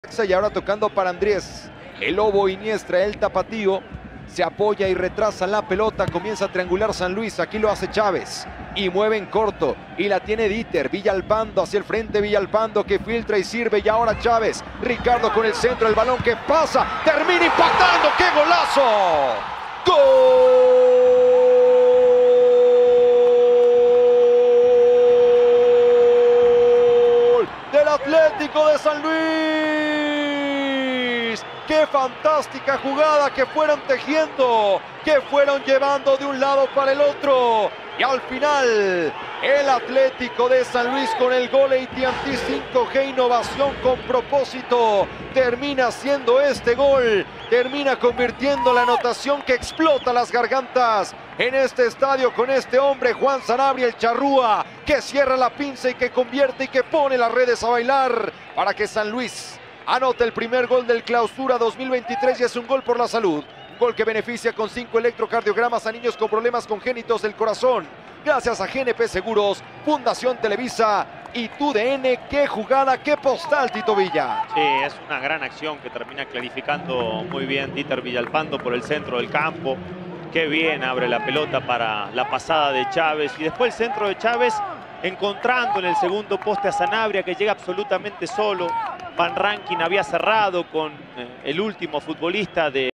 Gol de Juan Sanabria | Necaxa 0-1 San Luis | Liga BBVA MX - Clausura 2023 - Jornada 1
El PRIMER G⚽⚽⚽L del Clausura2023 es Charrúa 🇺🇾; es de SAN LUIS.

Gran remate de Juan Sanabria 🇺🇾 para abrir el marcador.

Necaxa 0-1 San Luis